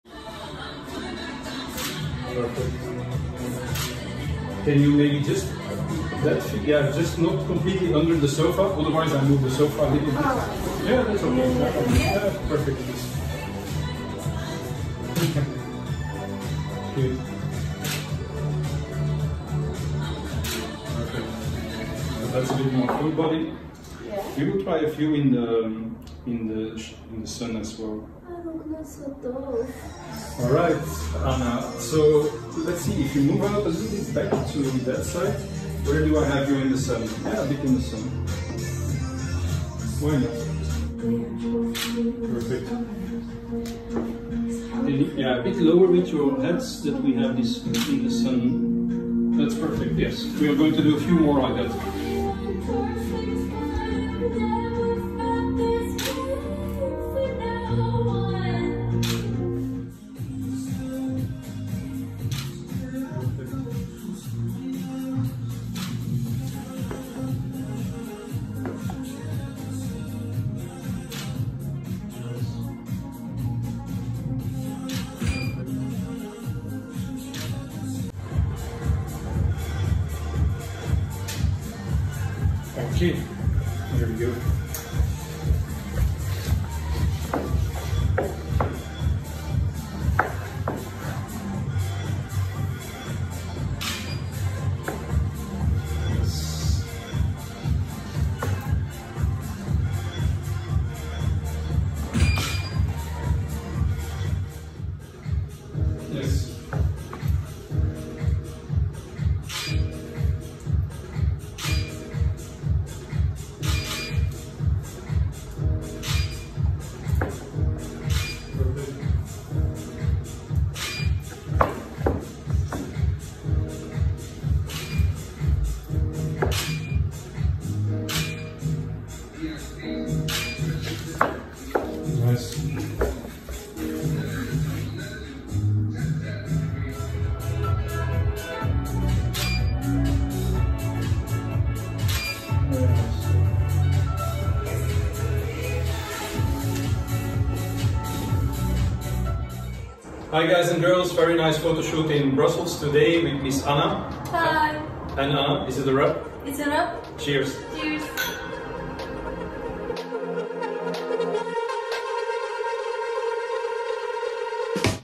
Anna Sokolova - Behind The Scenes
Behind the scenes at a boudoir photoshoot with Anna Sokolova, model from Ukraine.